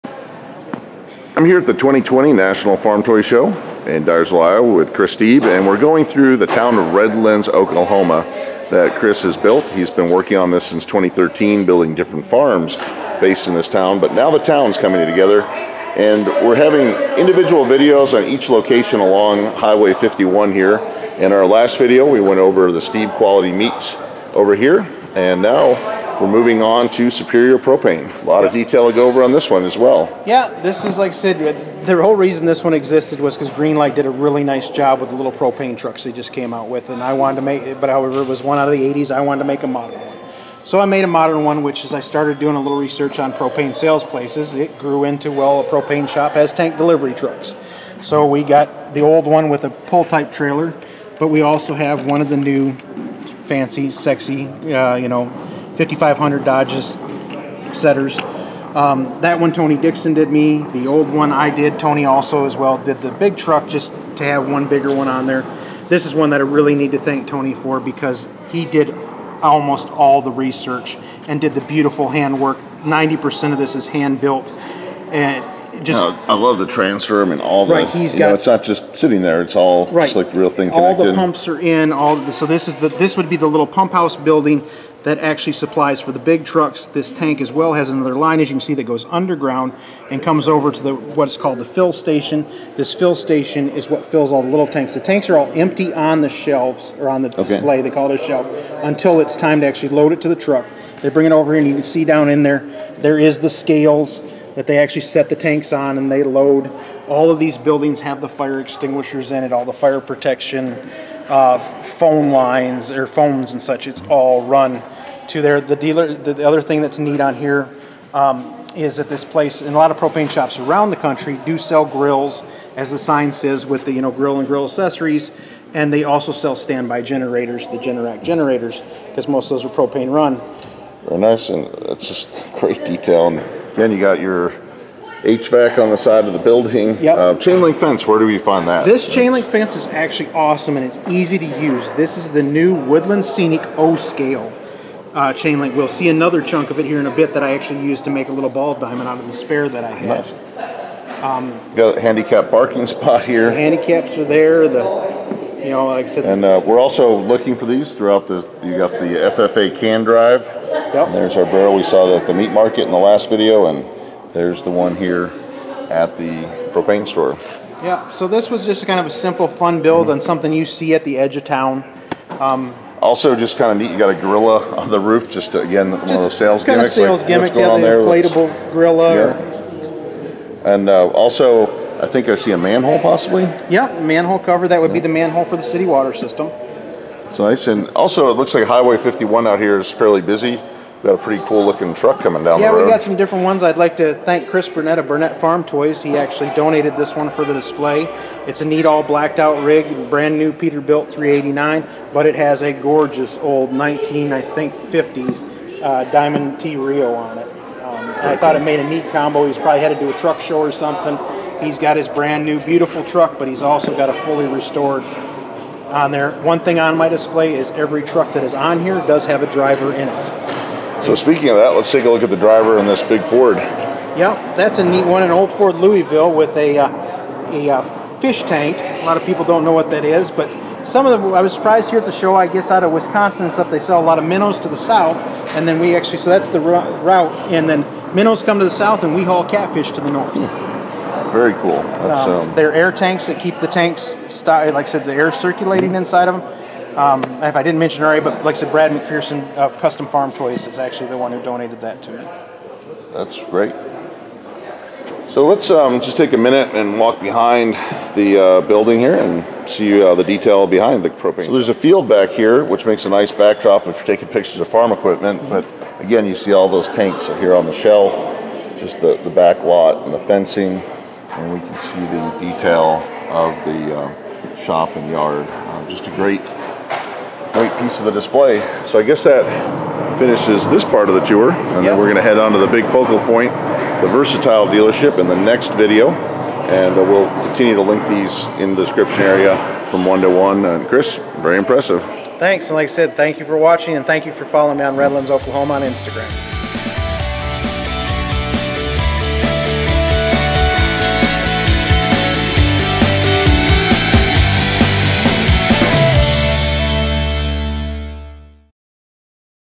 Red Lands Oklahoma Farm Display Tour Part II
Toy Tractor Times talks with Chris Steeb about his Red Lands, OK display at the 2020 National Farm Toy Show.  Chris has built a town based around a Versatile Tractor Dealer and farm.   In this video we continue an in-depth tour of this high detail farm display.  See Part I of the tour